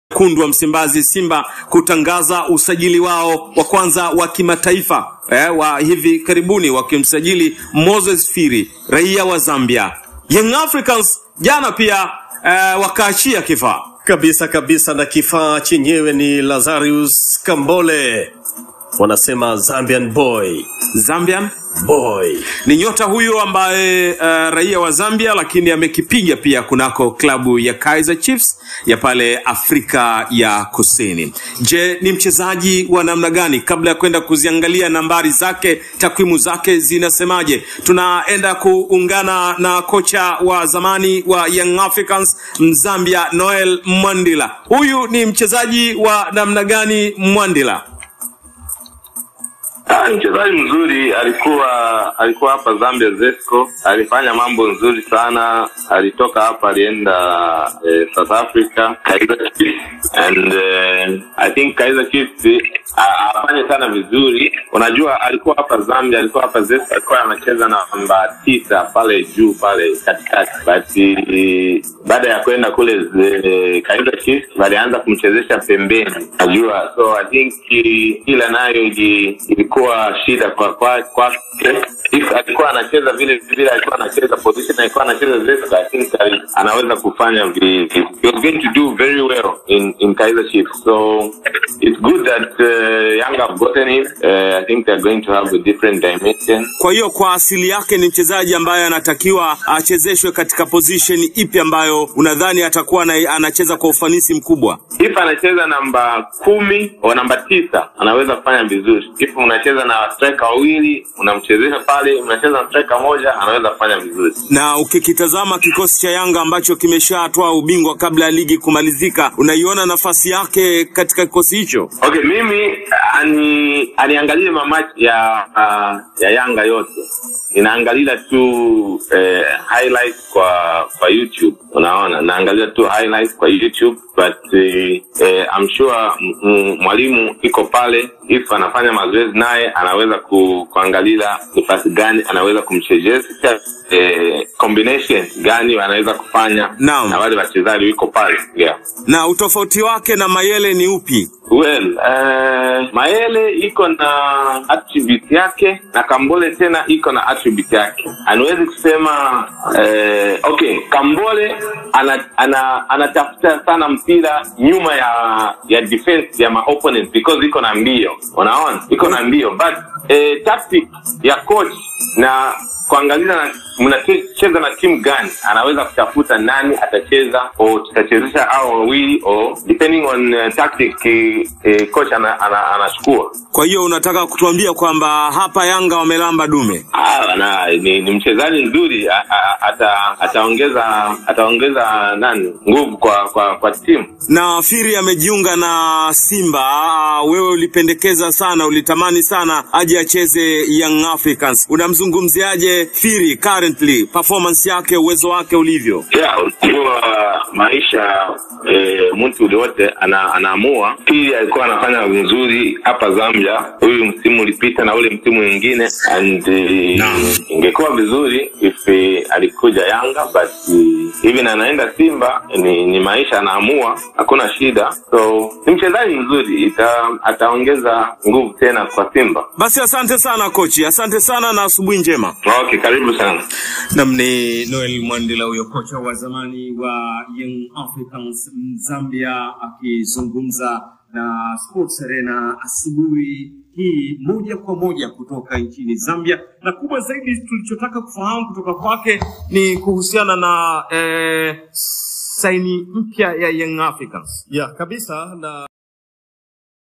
Yanga wamepata mtu na nusu Kocha wa zamani wa yanga
anaweza kuziba nafasi ya mayele kama ataondoka